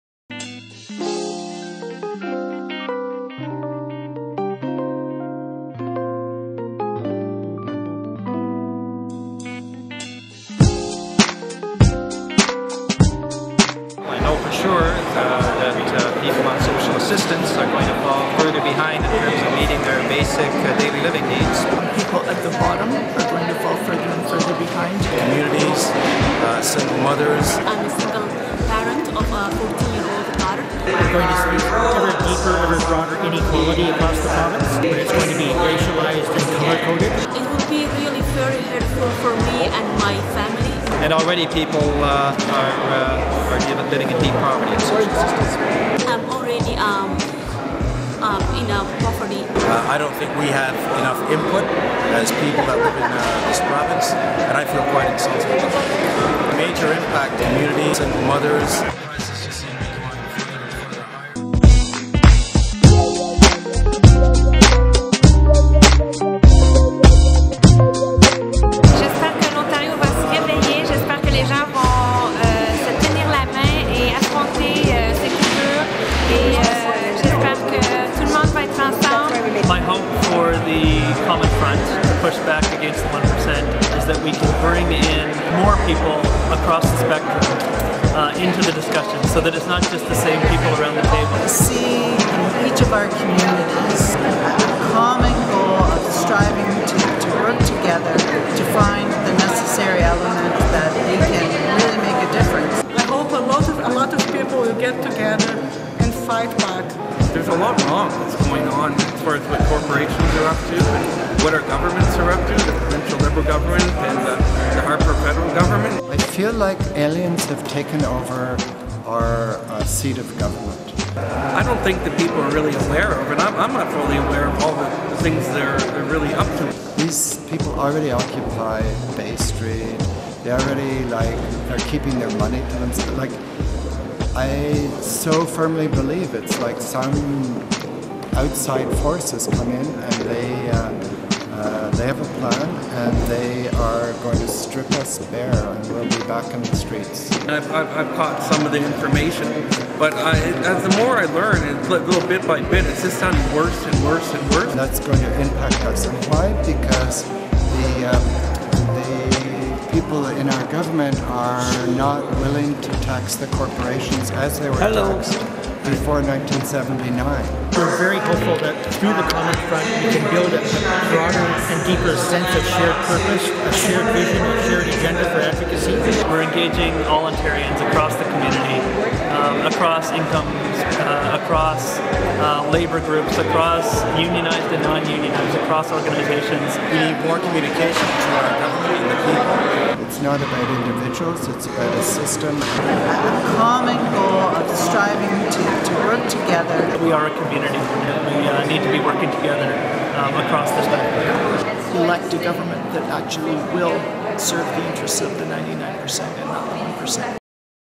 "We Are Ontario"! Ontarians tell McGuinty why cuts to jobs and services hurt the majority!
Ontarians from all walks of life are coming together in a Common Front of labour unions and community groups to oppose cuts to jobs and services that will hurt the majority. We must demand a better Ontario that is fair for everyone. Interviews conducted by the NIA Centre for the Arts.